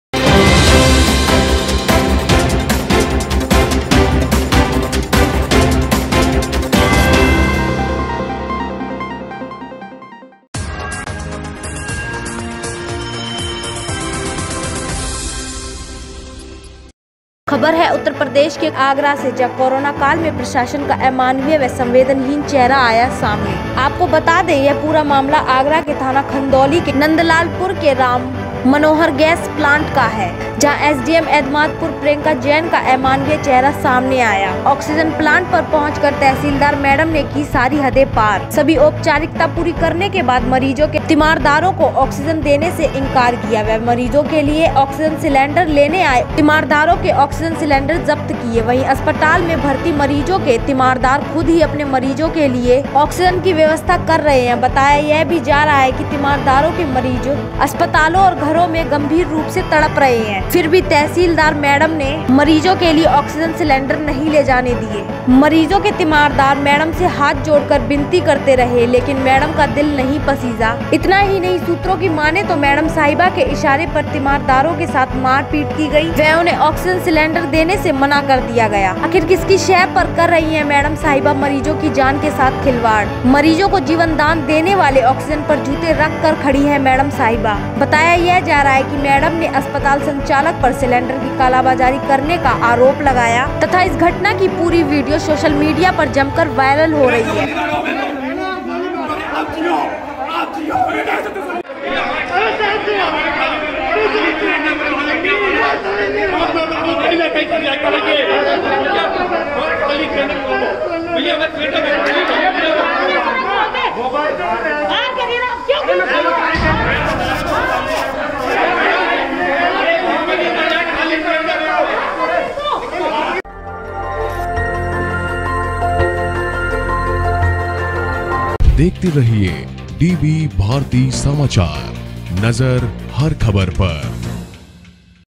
आगरा उ.प्र. खबर
शहर में लगातार चरमरा रही चिकित्सा व्यवस्था।

लगातार डेंजर जोन की ओर बढ़ रहा है आगरा

ऑक्सीजन की किल्लत से परेशान हो रहे  मरीज

रिक्शे पर मरीजों को ले जा रहे  तीमारदार

एसडीएम एत्मादपुर प्रियंका जैन का अमानवीय चेहरा

तीमारदारों को ऑक्सीजन देने से किया इनकार।

तीमारदारों के ऑक्सीजन सिलेंडर किये जब्त

 तीमारदार कर रहे ऑक्सीजन की व्यवस्था।

अस्पतालों  में तड़प रहे तीमारदार मरीज

तहसीलदार मेडम ने की सारी हदें की पार।

 मेडम ने नही ले जाने दिए ऑक्सीजन सिलेंडर

तीमारदार हाथ जोड़ कर करते रहे बिनती,

तीमारदारों के साथ की गयी मारपीट।
मेडम साहिबा ने मरीजो की जान के साथ किया खिलवाड़

ऑक्सीजन पर जूते रखकर खड़ी है मेडम साहिबा

नंदलालपुरा के राम मनोहर गैस प्लांट का मामला।